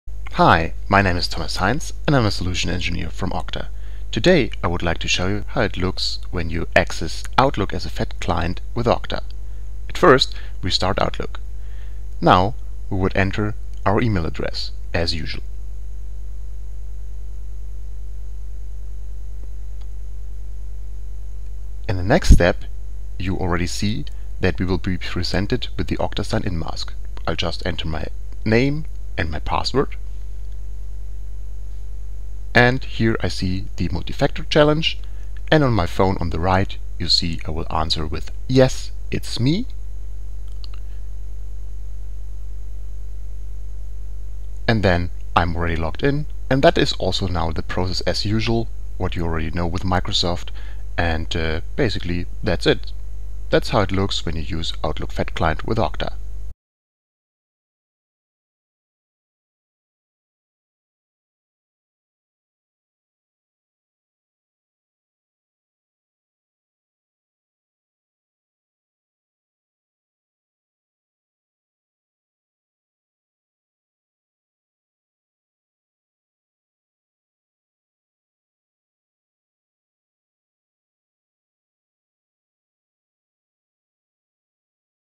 Outlook Fat Client / Outlook Desktop with Okta Login
Short demo how the Outlook Fat Client login flow looks when Okta is the Identity Provider.